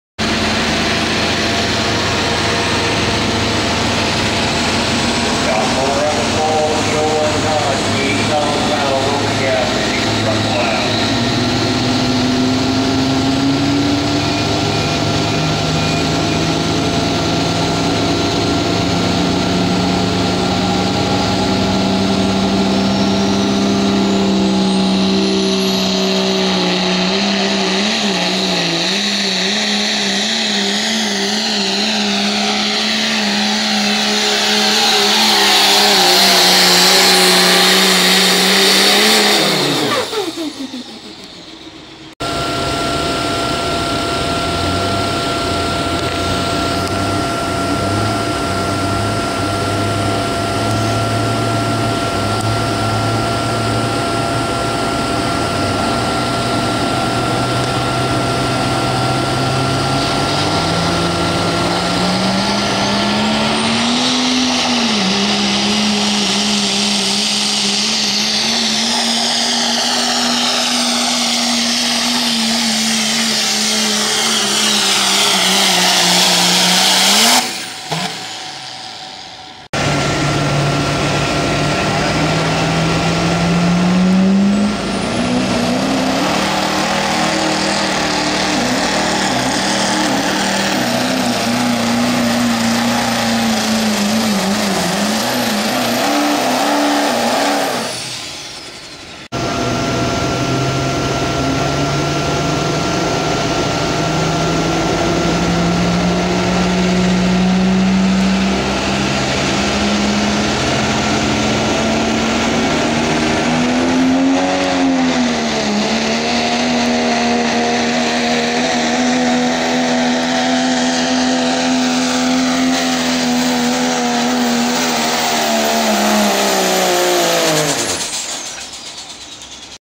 Henry Open.wmv
Most of the open class trucks from Henry, IL 2010.
I took 2nd place.